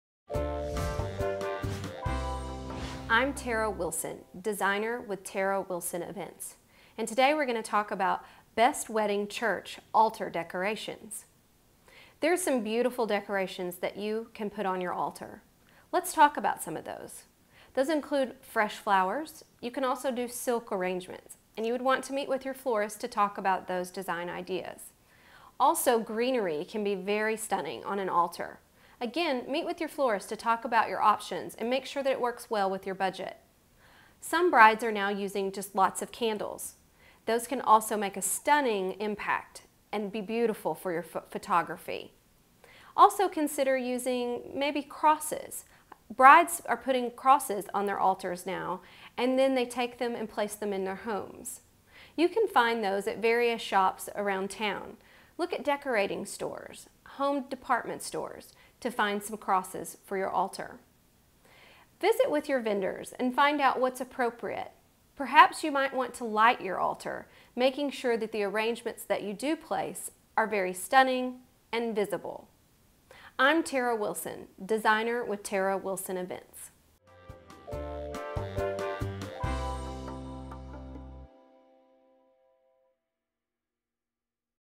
Best Wedding Church Altar Decorations
Church altars can be decorated for a wedding using fresh flowers, silk flowers or an array of candles. Discover the types of altar decorations allowed by the church with helpful tips from a professional event coordinator in this video on planning a wedding.

Expert: Tara Wilson
Bio: Tara Wilson is the owner of Tara Wilson Events, a full-service event company committed to providing creative, well-executed and memorable events for individual and corporate clients.

Opening photo by Cunningham Photography